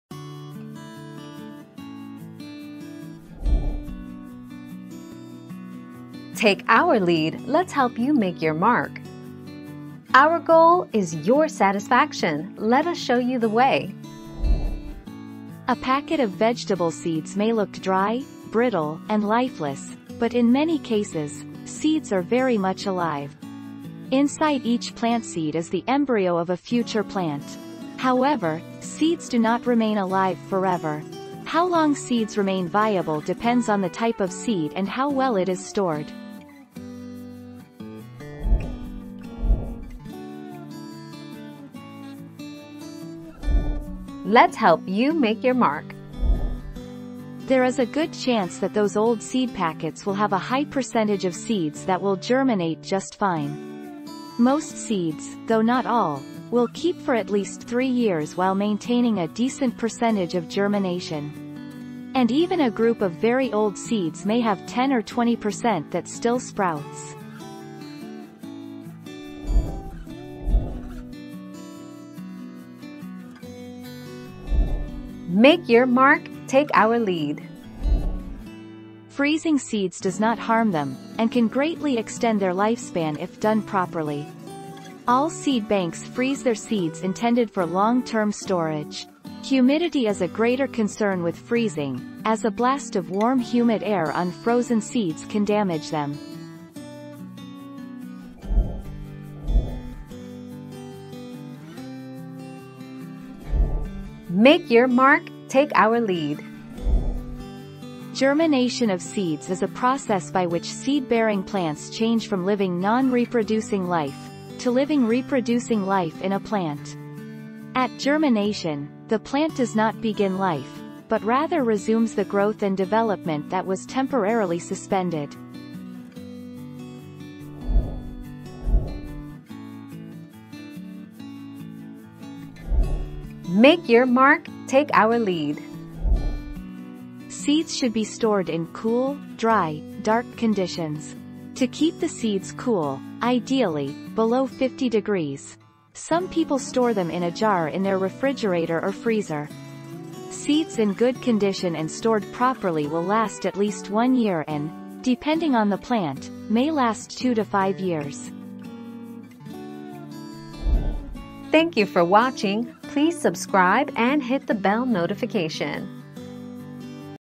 Do seeds die?
00:00 - Do seeds die?
00:40 - Will 20 year old seeds grow?
01:11 - Do seeds die if they freeze?
01:40 - Does a seed die during germination?
02:09 - Can seeds last forever?

Laura S. Harris (2021, April 4.) Do seeds die?
    AskAbout.video/articles/Do-seeds-die-208860